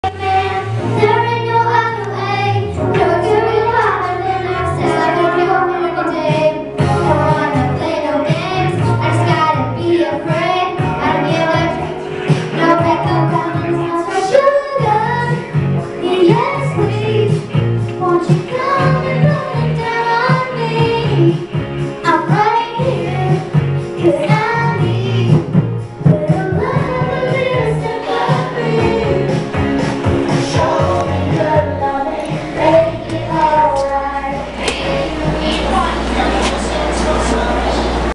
Jefferson Teen Connection- Girls Singing Karaoke
Girls singing Karaoke at Jefferson Teen Connection.